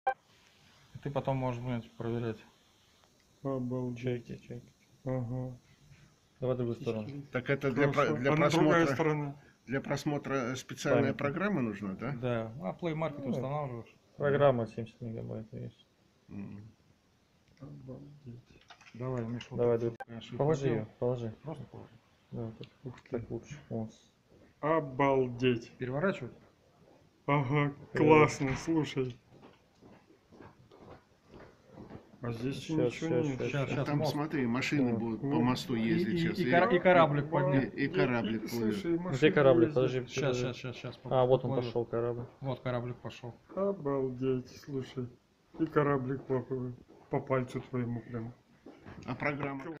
Новые российские купюры 200 и 2000 рублей
Решили посмотреть разрекламированную защиту новых российских купюр